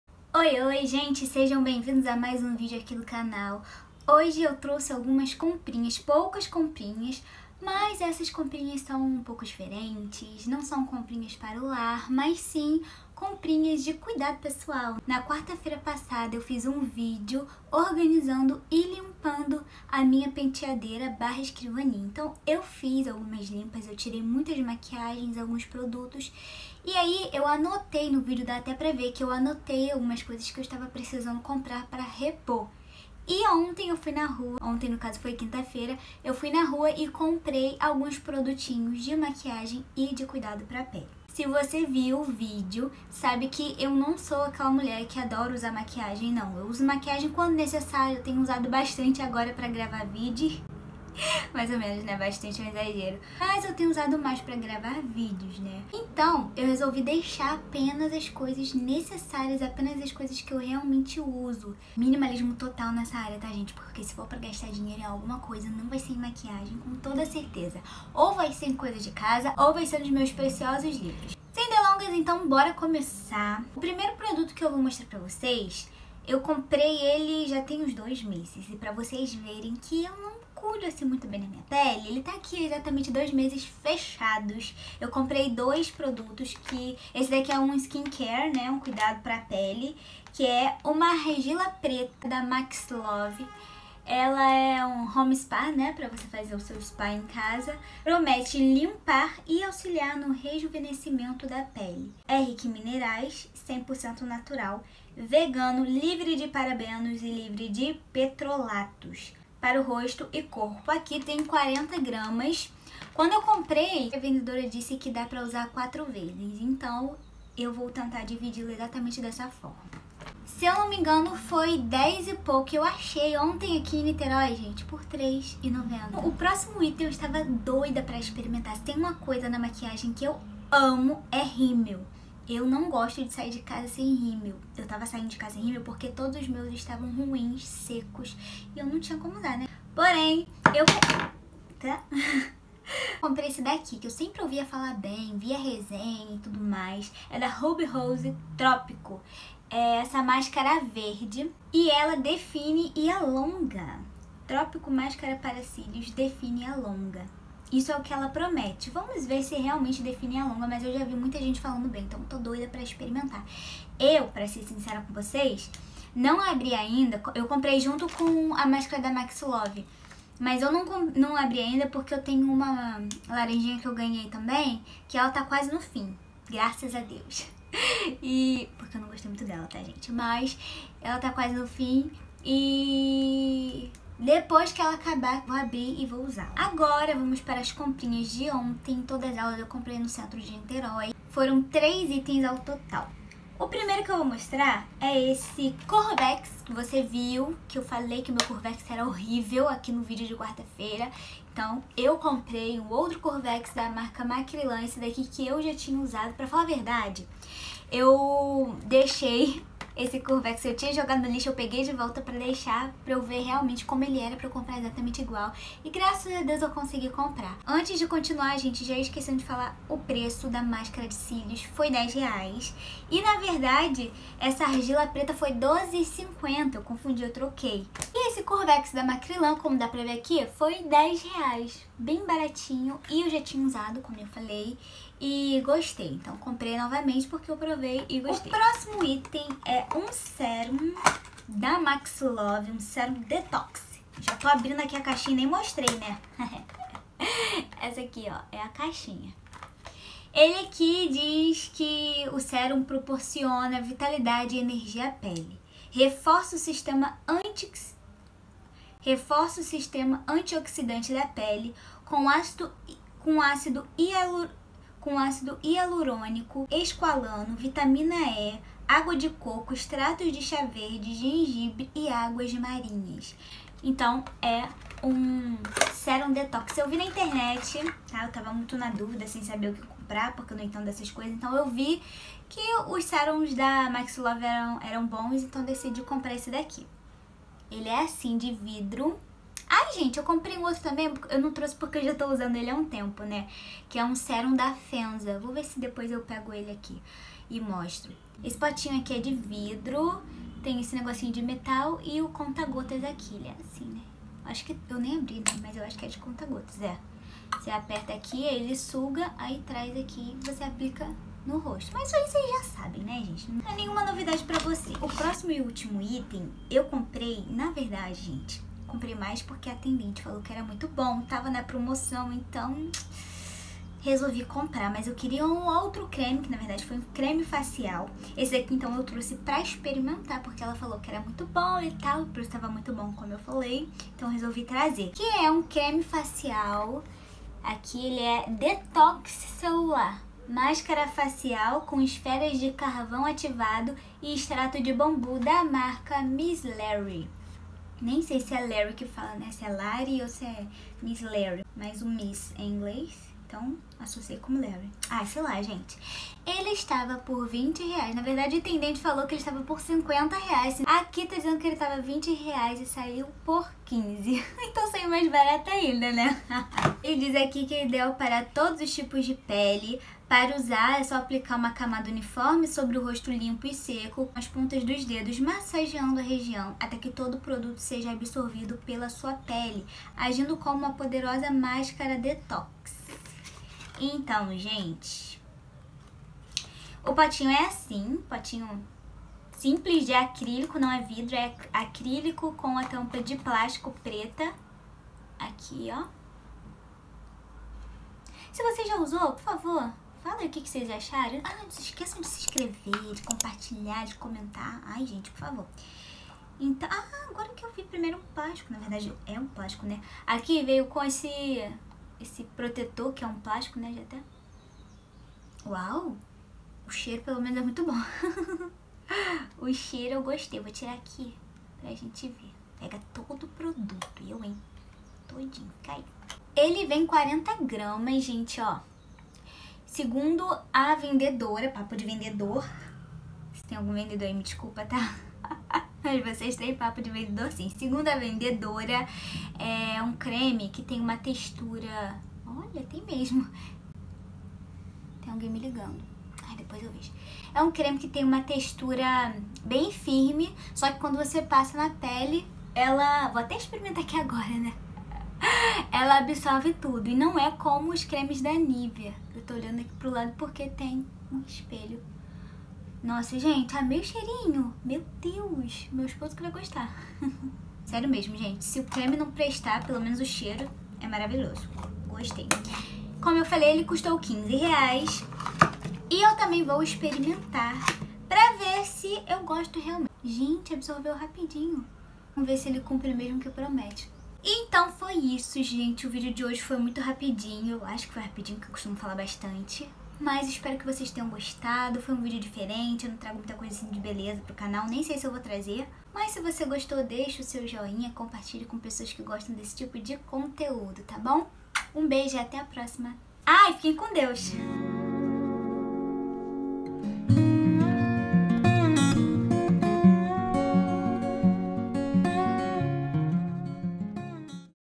COMPRINHAS DE MAQUIAGEM E PRODUTOS DE BELEZA | Comprei itens de Make Up E Cosméticos Baratinhos
OI, OI, GENTE!

No vídeo de hoje mostrei a vocês as comprinhas que fiz na quinta feira passada, logo após limpar e organizar a penteadeira/escrivaninha do meu quarto. Confira Aqui!
Como fiz a limpa, literalmente, nas minhas maquiagens e produtos de beleza e cosméticos, precisei comprar novos itens de make up e companhia... Trouxe apenas itens baratinhos e um mega achadinho!

Se você gosta desse tipo de conteúdo, não deixe de conferir este vídeo!
Sem delongas, BORA LÁ!

Att.
Nathalia Ayres 😘